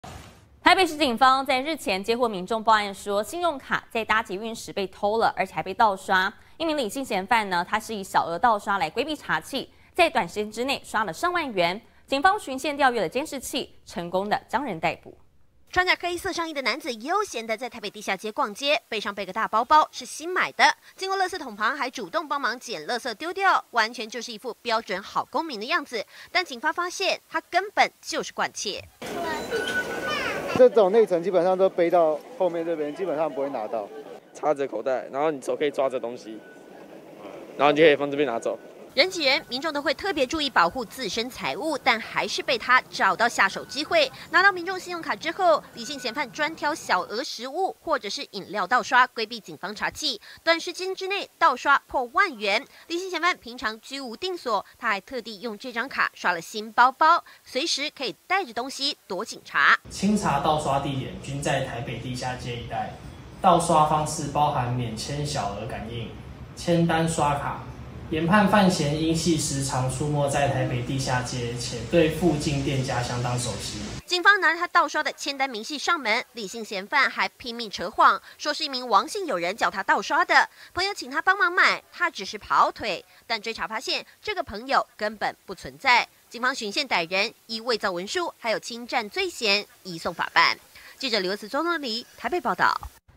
撿垃圾裝好公民 慣竊捷運偷信用卡盜刷
搭捷運人多要小心扒手慣竊! 台北市警中正一分局，日前接獲民眾報案，信用卡在搭捷運時被偷而且被盜刷，李姓嫌犯以小額盜刷規避查緝，短時間之內刷爆上萬元，警方循線調閱監視器逮捕嫌犯。